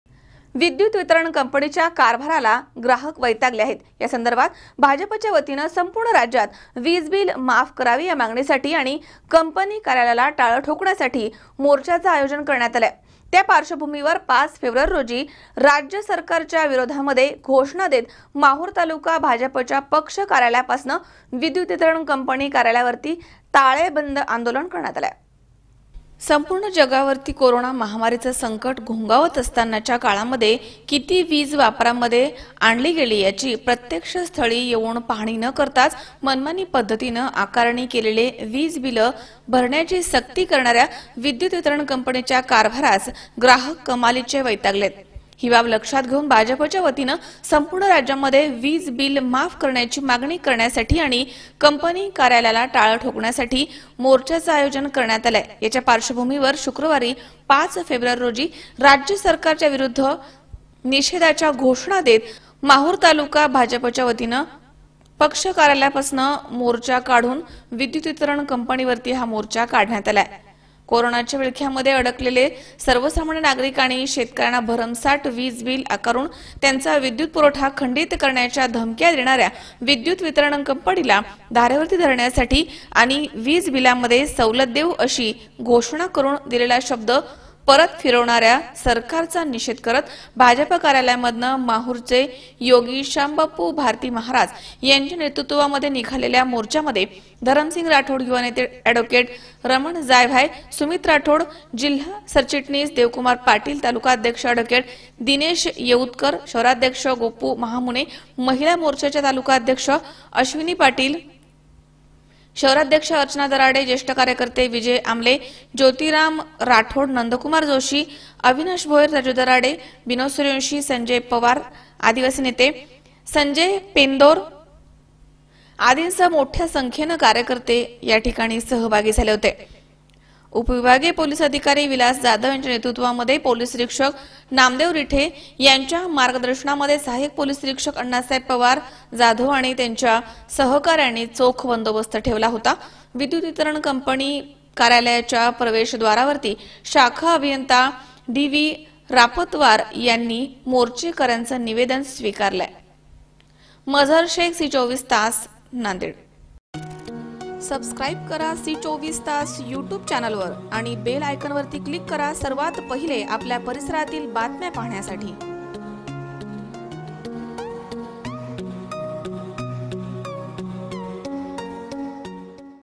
नांदेड - माहूर भाजप तर्फे वीज बिल माफीसाठी ताळेबंद आंदोलन.
Category
News & Politics